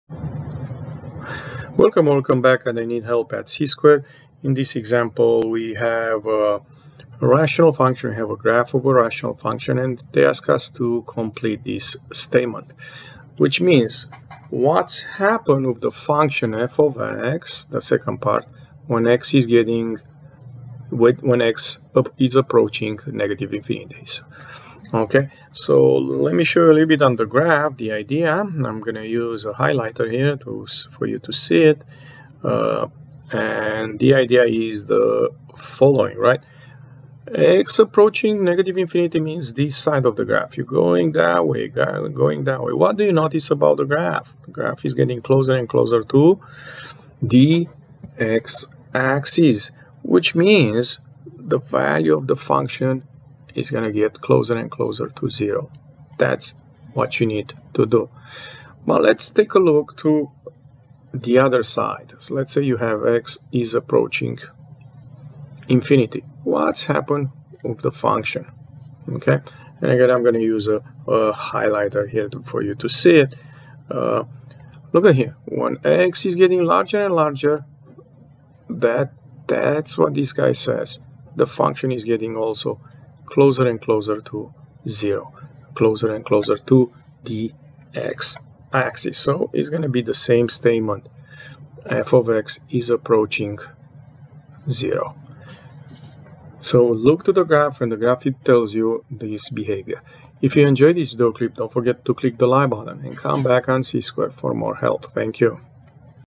INH: Rational Functions: Behavior #3
This is an example of describing the behavior of a rational function.